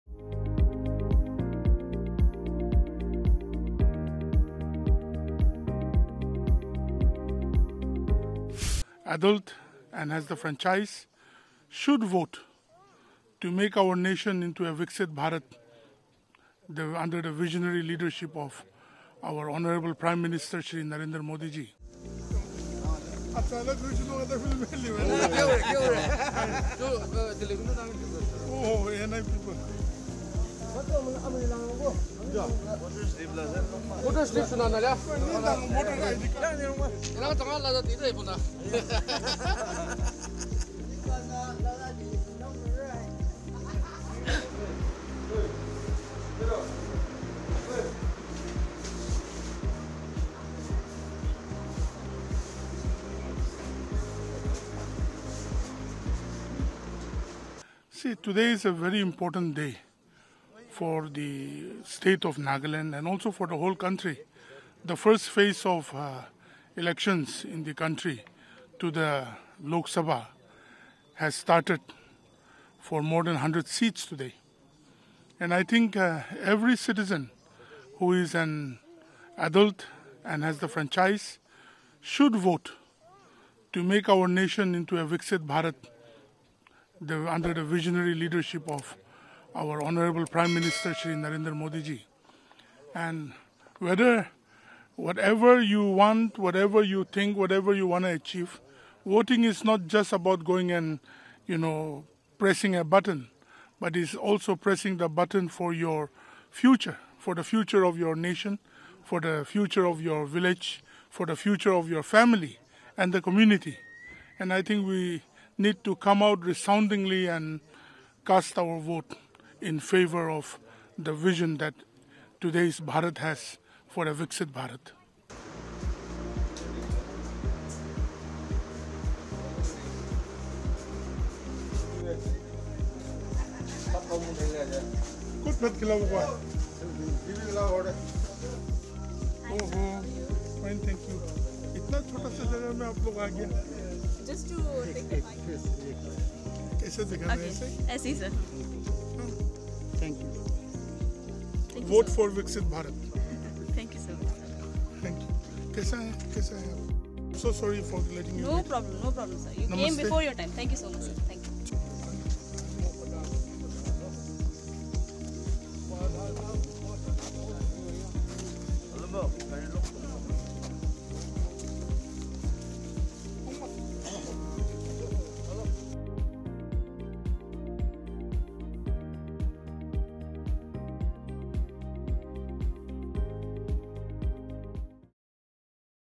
“Vote for Viksit Bharat…” Nagaland Minister Temjen Imna after casting vote
Nagaland Tourism and Higher Education Minister and BJP leader Temjen Imna Along cast his vote at a polling station in Alongtaki, Mokokchung. After casting the vote, Along said that every citizen should vote to transform the nation into 'Viksit Bharat' under the leadership of PM Modi. While speaking to ANI, Nagaland minister Temjen Imna Along said, "Today is a very important day for Nagaland and the entire country as the first phase of voting for Lok Sabha elections has started today. Every citizen should vote to make our nation into 'Viksit Bharat' under the leadership of PM Modi. We need to come out and cast our vote in favour of the vision that today's Bharat has for a 'Viksit Bharat'."